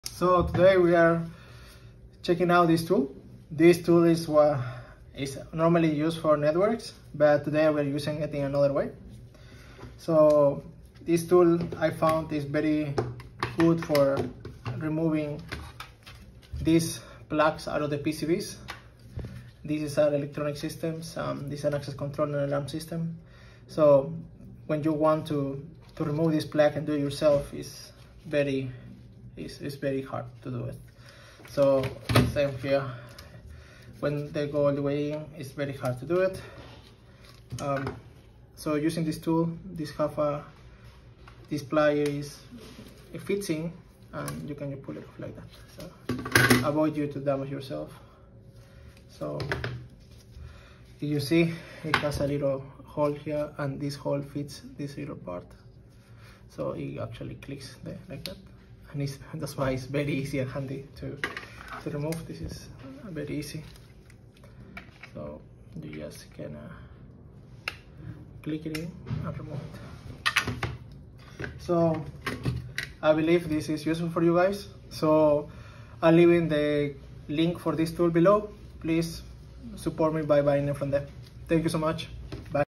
This Network Tool Makes Removing PCB Plugs Effortless
Tired of hurting your fingers trying to remove tight PCB terminal plugs from security systems? In this video, we reveal a simple tool — originally made for network cable management — that makes unplugging those stubborn terminals fast, easy, and painless. It’s a game-changer for technicians working with access control panels, alarm systems, or any tight wiring jobs.

🔧 See how this underrated tool saves time and frustration.

💡 Perfect for installers, techs, and anyone working with PCBs.